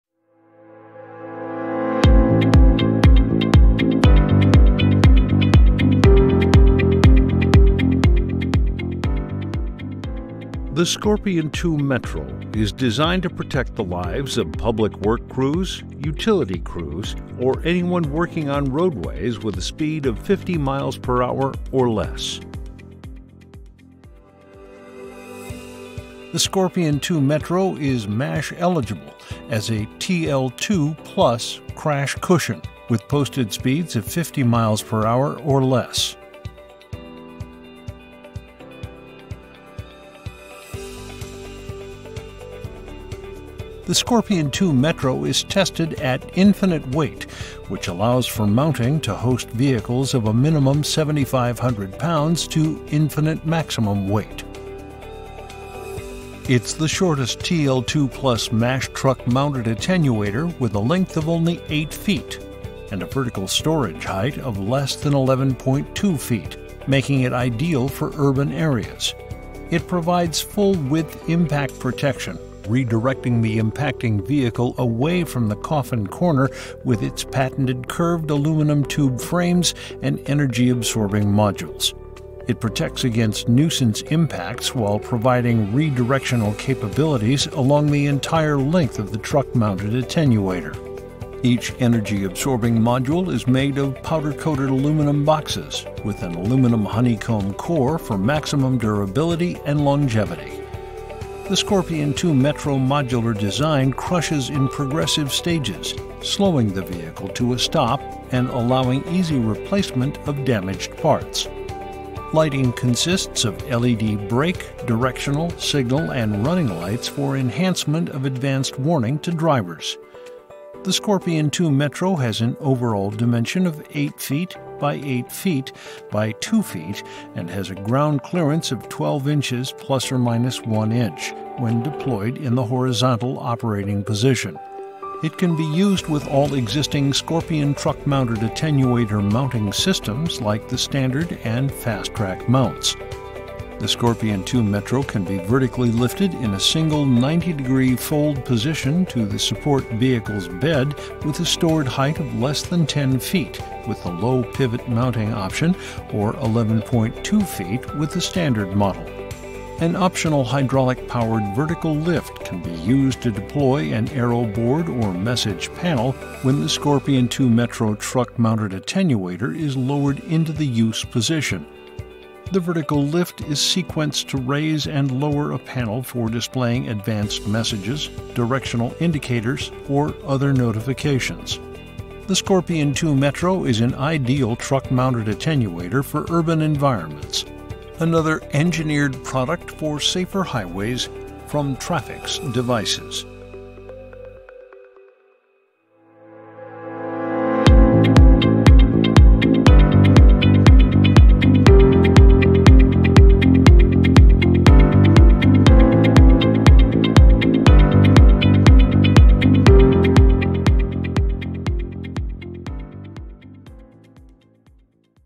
Scorpion II® Metro Truck Mounted Attenuator — TrafFix Devices
The Scorpion II METRO is the ideal TMA for cities, counties, and urban districts with posted speeds of 50 mph (80 kph) or less.

The Scorpion II Metro is the shortest TL-2 Plus MASH TMA on the market with a length of only 8 ft (2.44 m), and a vertical storage height of less than 11.2 ft (3.4m), making it ideal for urban areas. The Scorpion II Metro provides full width impact protection by redirecting the impacting vehicle away from the "coffin corner" with its patented curved aluminum tube frames, and energy absorbing modules. The curved aluminum tubes provide protection against nuisance impacts while providing re-directional capabilities along the entire length of the TMA. Each energy absorbing module is made powder coated aluminum boxes with an aluminum honeycomb core for maximum durability and longevity.